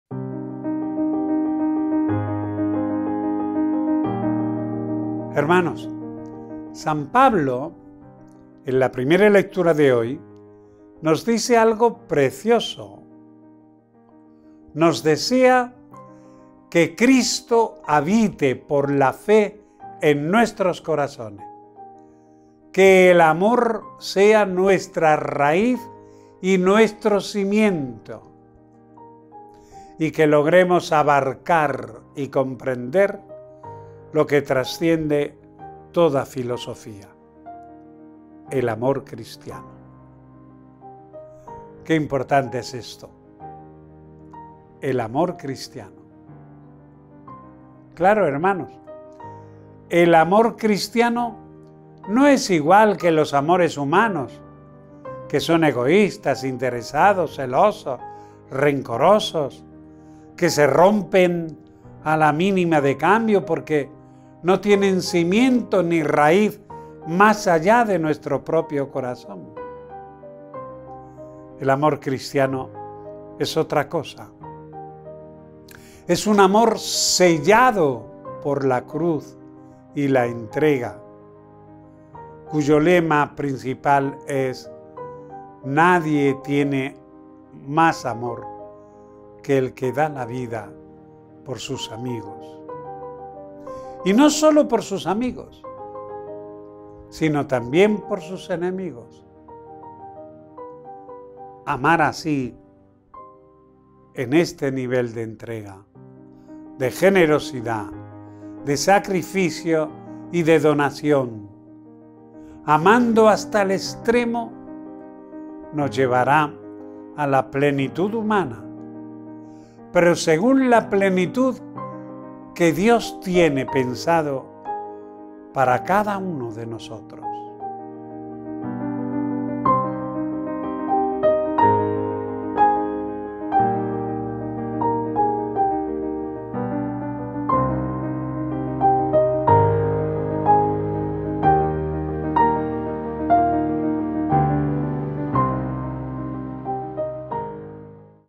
20 - 10 - 2022 | Que el amor sea nuestra raíz y nuestro cimiento
Hermanos, San Pablo en la primera lectura de hoy nos dice algo precioso, nos desea “que Cristo habite por la fe en nuestros corazones, que el amor sea nuestra raíz y nuestro cimiento, y que logremos abarcar y comprender lo que trasciende toda filosofía, el amor cristiano”. ¡Qué importante es esto! El amor cristiano. Claro hermanos, el amor cristiano no es igual que los amores humanos, que son egoístas, interesados, celosos, rencorosos, que se rompen a la mínima de cambio porque no tienen cimiento ni raíz más allá de nuestro propio corazón. El amor cristiano es otra cosa, es un amor sellado por la cruz y la entrega, cuyo lema principal es “nadie tiene más amor que el que da la vida por sus amigos”, y no sólo por sus amigos, sino también por sus enemigos. Amar así, en este nivel de entrega, generosidad, sacrificio y donación, amando hasta el extremo, nos llevará a la plenitud humana, pero según la plenitud que Dios tiene pensado para cada uno de nosotros.